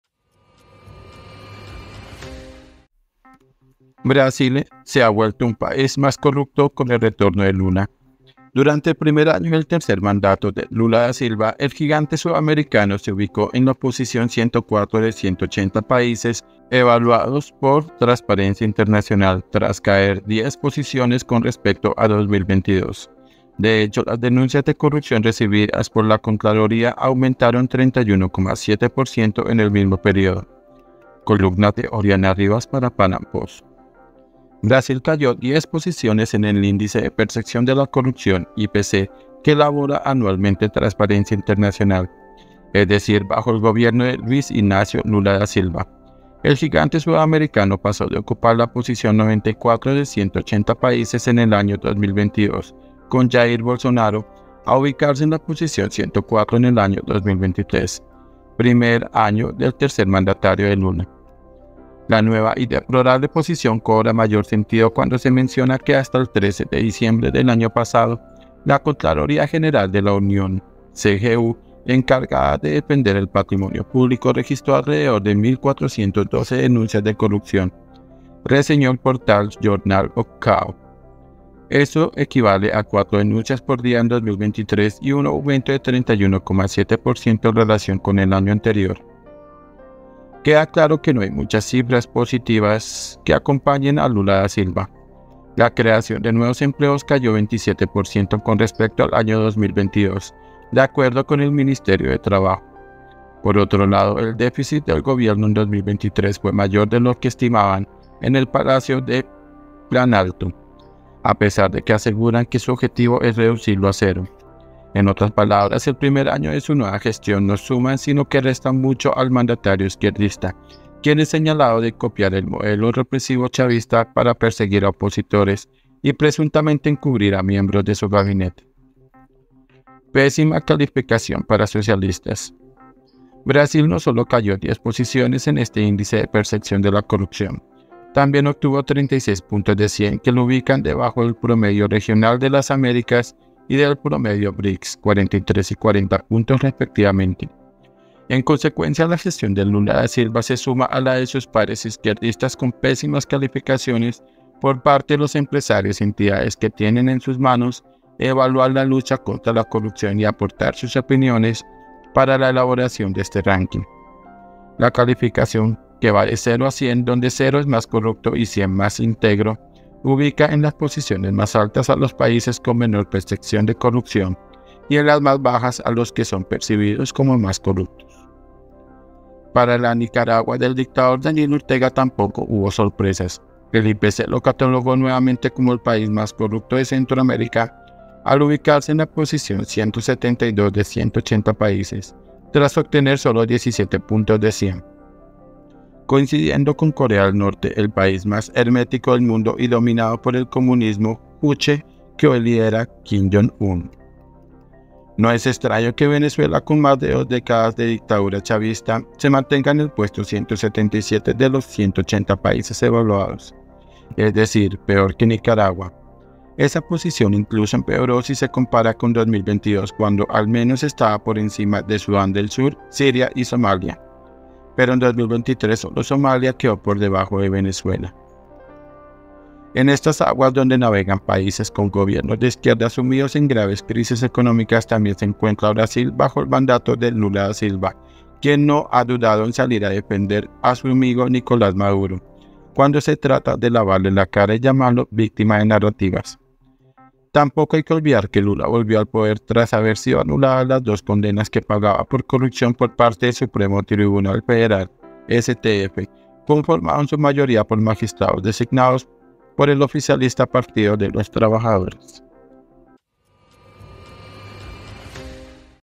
🛑 BRASIL SE HA VUELTO UN PAÍS MÁS CORRUPTO CON EL RETORNO DE LULA
Durante el primer año del tercer mandato de Lula da Silva, el gigante sudamericano se ubicó en la posición 104 de 180 países evaluados por Transparencia Internacional, tras caer 10 posicione con respecto a 2022. De hecho, las denuncias de corrupción recibidas por la Contraloría aumentaron 31,7 % en el mismo periodo.
POR ORIANA RIVAS PARA PANAM POST